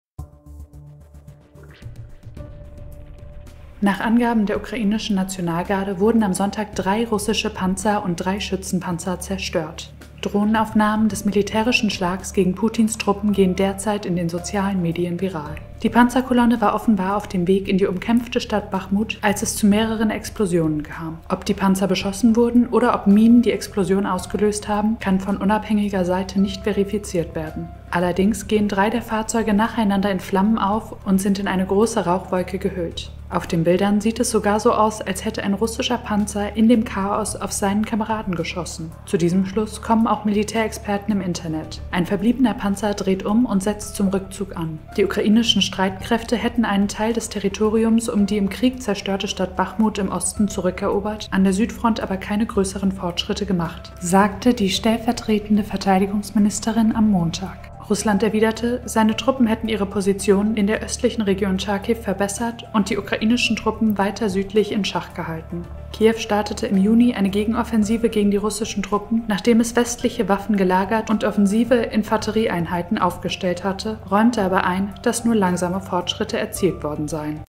Ukrainer stoppen Russen-Kolonne - dann bricht das Chaos los
Nach Angaben der ukrainischen Nationalgarde wurden am Sonntag drei russische Panzer und drei Schützenpanzer zerstört. Drohnenaufnahmen des militärischen Schlags gegen Putins Truppen gehen aktuell in den sozialen Medien viral.

Die Panzerkolonne war offenbar auf dem Weg in die umkämpfte Stadt Bachmut, als es zu mehreren Explosionen kam. Ob die Panzer beschossen wurden oder ob Minen die Explosionen ausgelöst haben, kann von unabhängiger Seite nicht verifiziert werden.
Allerdings gehen drei der Fahrzeuge nacheinander in Flammen auf und sind in eine große Rauchwolke gehüllt. Auf den Bildern sieht es sogar so aus, als hätte ein russischer Panzer in dem Chaos auf seine Kameraden geschossen. Zu diesem Schluss kommen auch Militärexperten im Internet. Ein verbliebener Panzer dreht um und setzt zum Rückzug an.

Die ukrainischen Streitkräfte hätten einen Teil des Territoriums um die im Krieg zerstörte Stadt Bachmut im Osten zurückerobert, an der Südfront aber keine größeren Fortschritte gemacht, sagte die stellvertretende Verteidigungsministerin am Montag.
Russland erwiderte, seine Truppen hätten ihre Positionen in der östlichen Region Charkiw verbessert und die ukrainischen Truppen weiter südlich in Schach gehalten.
Kiew startete im Juni eine Gegenoffensive gegen die russischen Truppen, nachdem es westliche Waffen gelagert und offensive Infanterieeinheiten aufgestellt hatte, räumte aber ein, dass nur langsame Fortschritte erzielt worden seien.